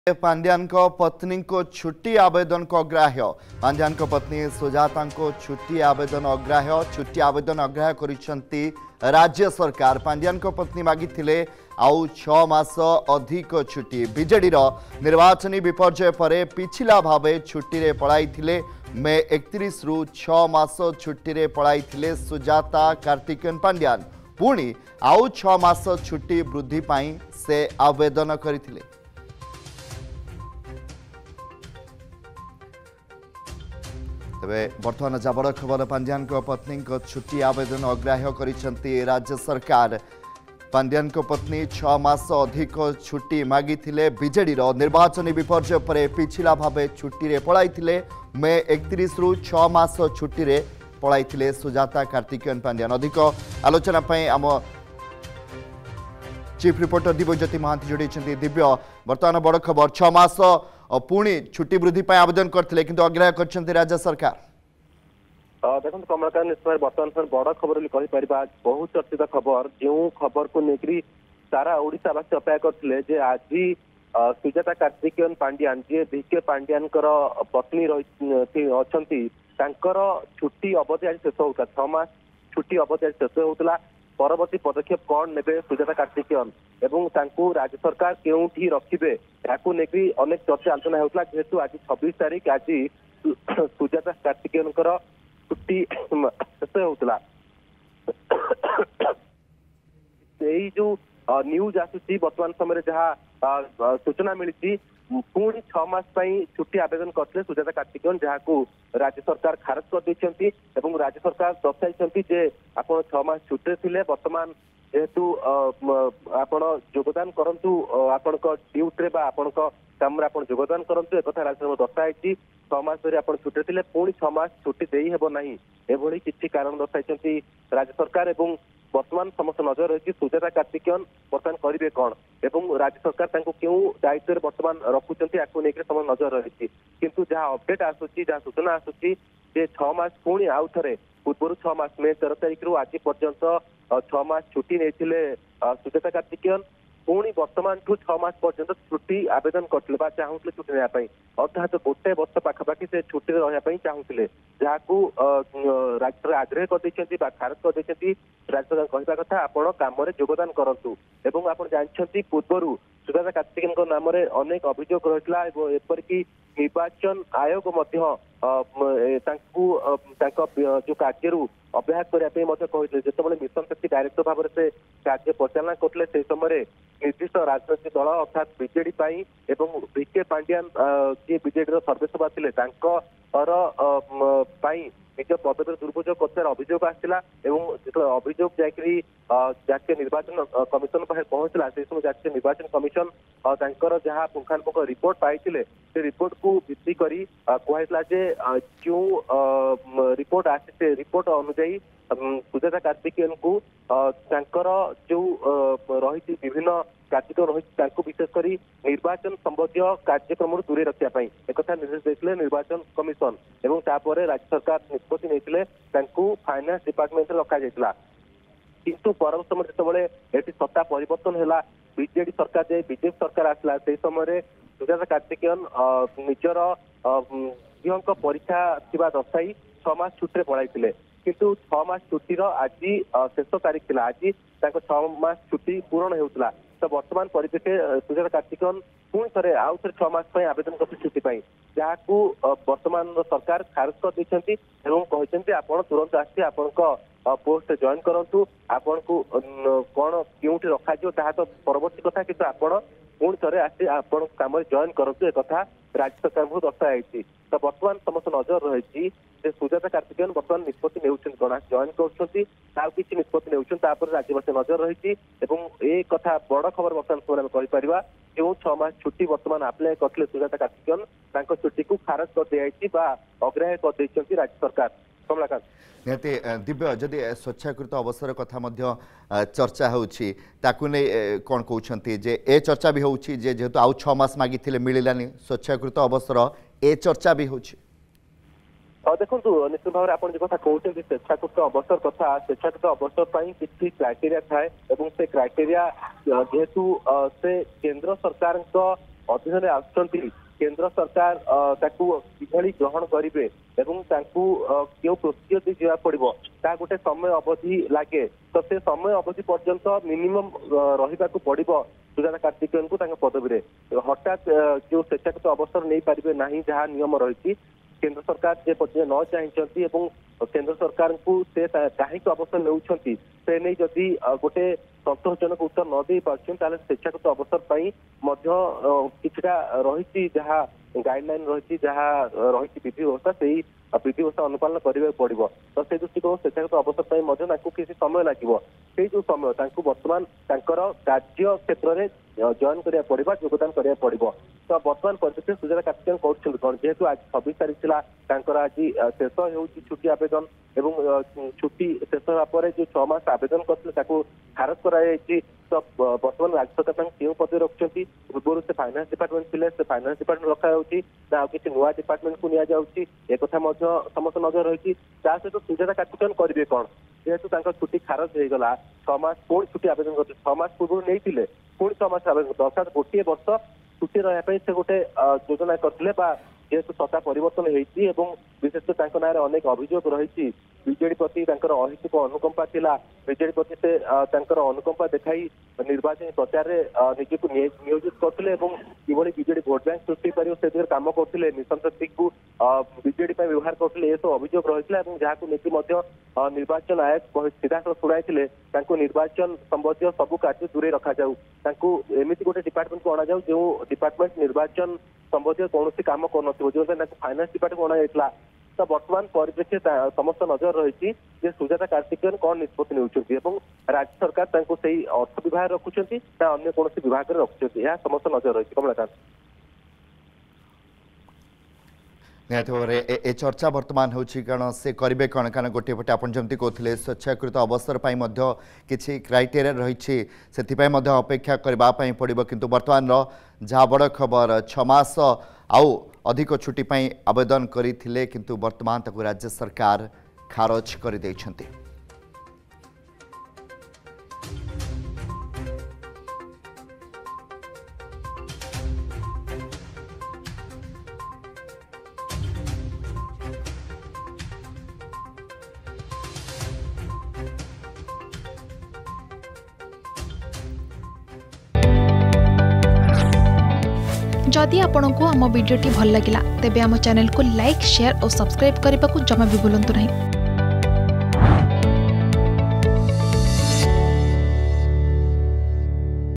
ପାଣ୍ଡିଆନଙ୍କ ପତ୍ନୀ ସୁଜାତାଙ୍କ ଛୁଟି ଆବେଦନ ଅଗ୍ରାହ୍ୟ | Sujata Karthikeyan Pandian
ପାଣ୍ଡିଆନଙ୍କ ପତ୍ନୀ ସୁଜାତାଙ୍କ ଛୁଟି ଆବେଦନ ଅଗ୍ରାହ୍ୟ । ଛୁଟି ଆବେଦନ ଅଗ୍ରାହ୍ୟ କଲେ ରାଜ୍ୟ ସରକାର